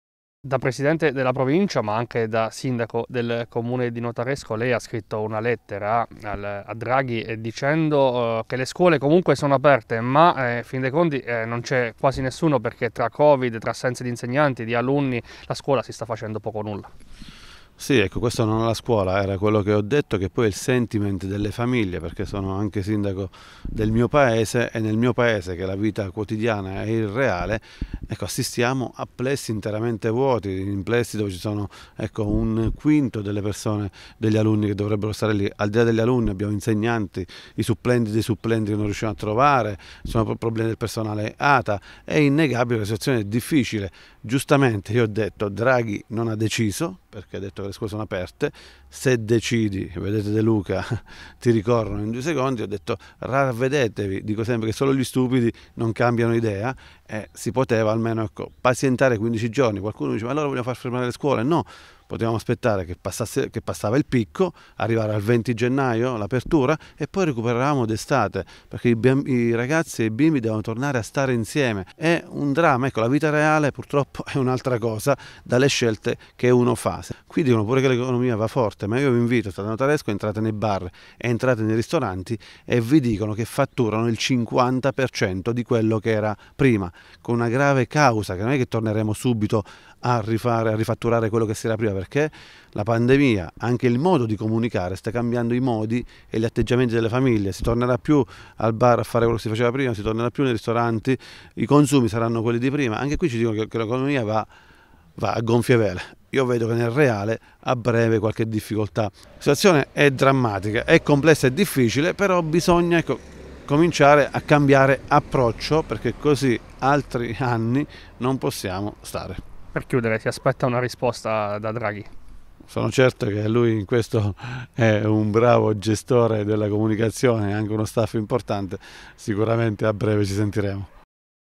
Di Bonaventura scrive a Draghi per il "problema" scuola
Ha sollecitato persino il presidente del Consiglio Mario Draghi il presidente della provincia di Teramo e sindaco di Notaresco Diego Di Bonaventura sulle scuole semivuote a causa dei contagi Covid.